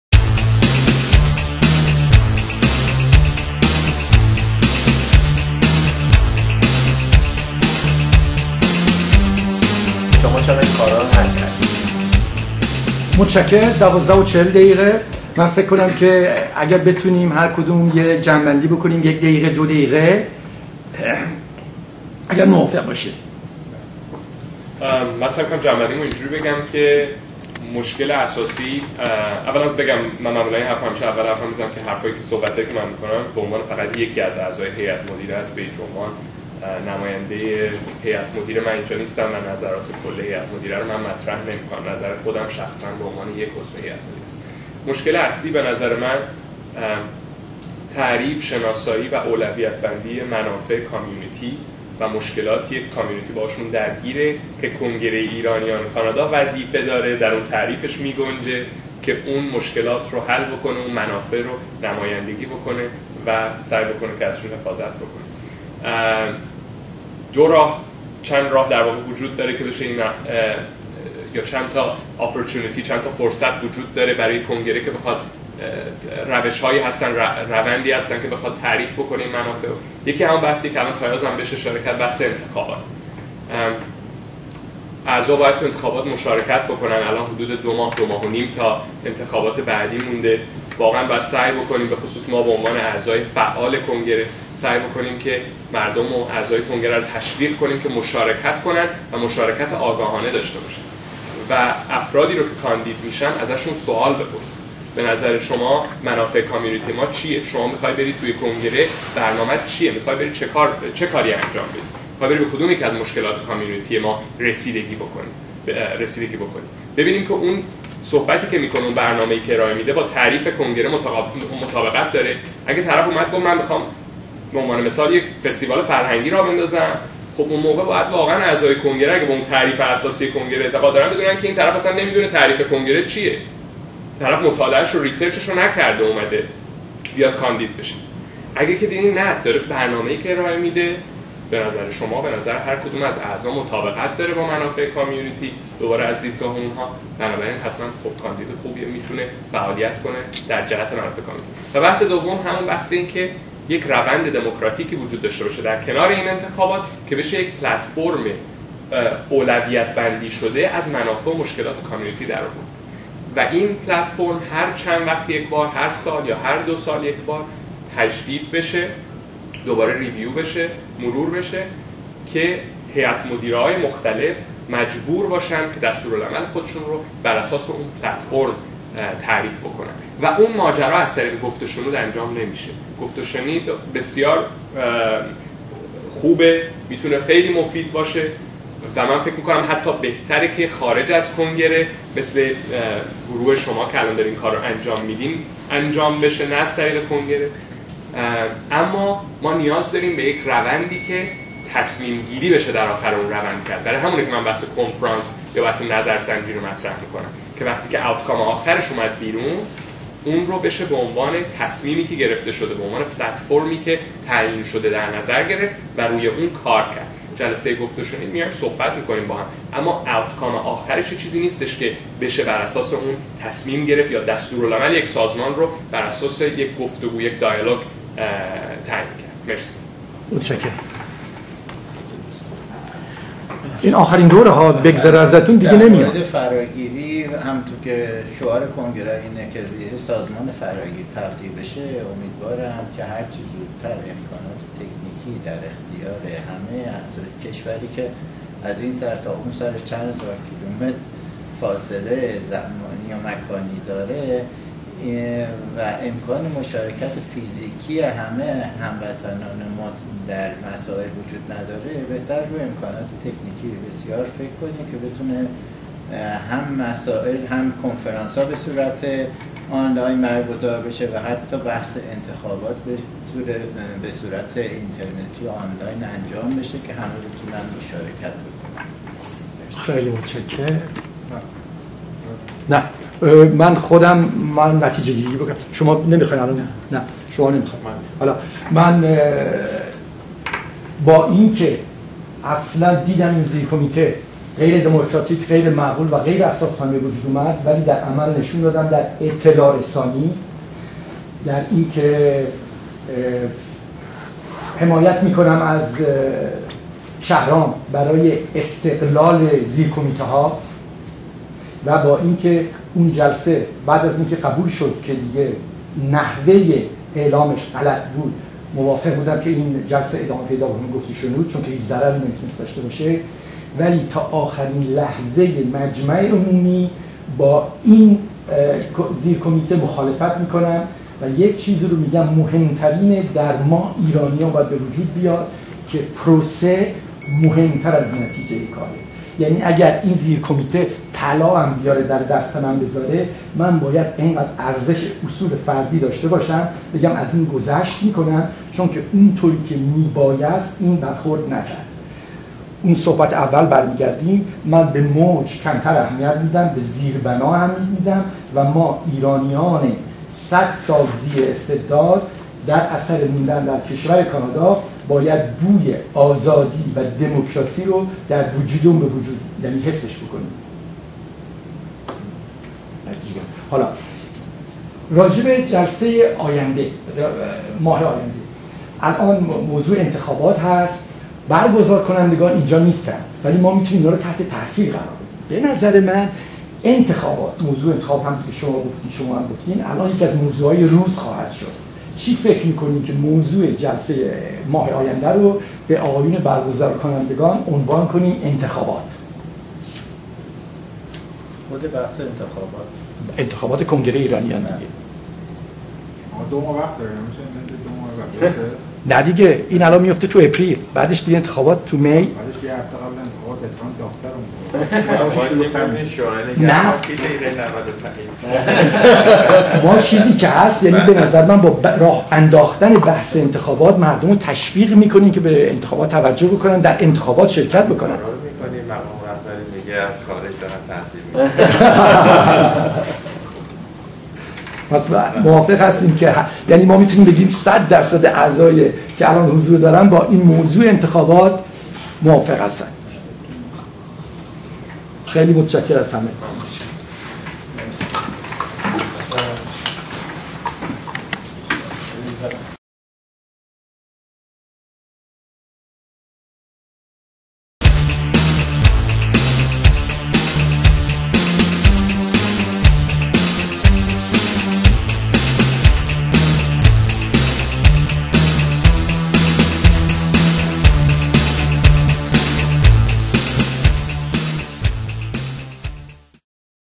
Goft - o - Shenid (22) - ICC By Law - Part 12/12 - Bijan Ahmadi - Amir Khosrozamiri - Medi Shams
Goft - o - Shenid held its 2nd gathering at North York Civic Centre on Sunday March 06, 2016. The title of the dialogue was " ICC By Law: Help or Obstacle"
"گفت و شنود" بیست و دومین گردهمائی خود را پیرامون "اساسنامه، مانع و یا راهگشای فعالیت های کنگره ایرانیان" را در روز  یکشنبه 7 فوریه در سیویک سنتر نورت یورک، برگزار کرد.